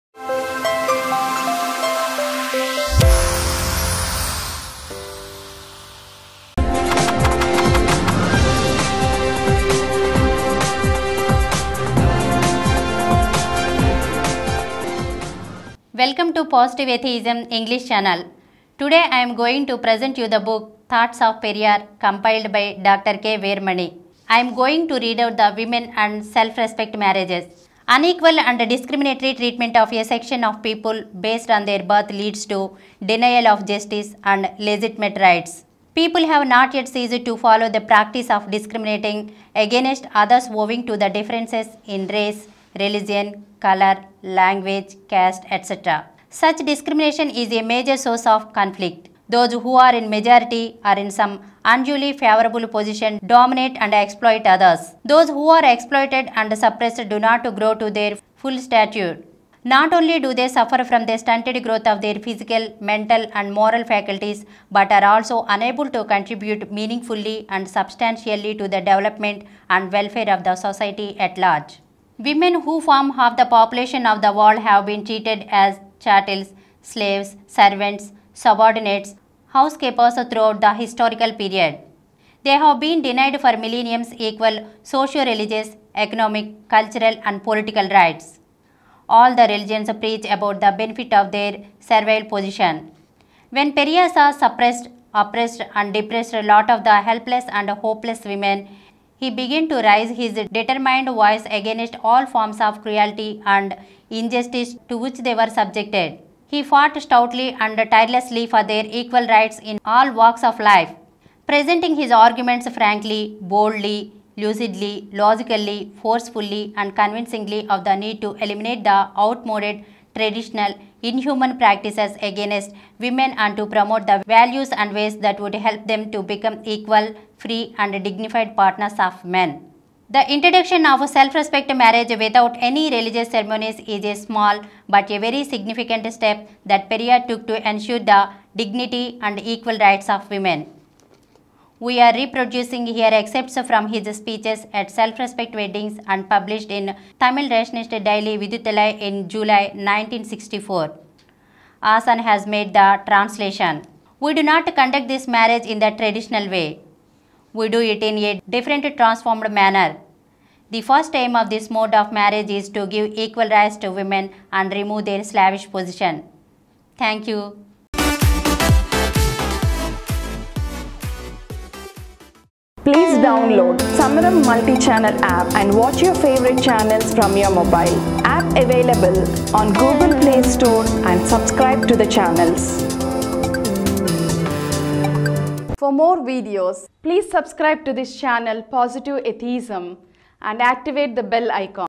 INTRODUCTION OF SELF RESPECT MARRIAGE BY PERIYAR (Episode -47) in Thoughts Of Periyar
INTRODUCTION OF SELF RESPECT MARRIAGE BY PERIYAR (Episode -47) in Thoughts Of Periyar Compiled by Dr K VEERAMANI. Read by Lakshmi Prameela in Positive Atheism English Channel.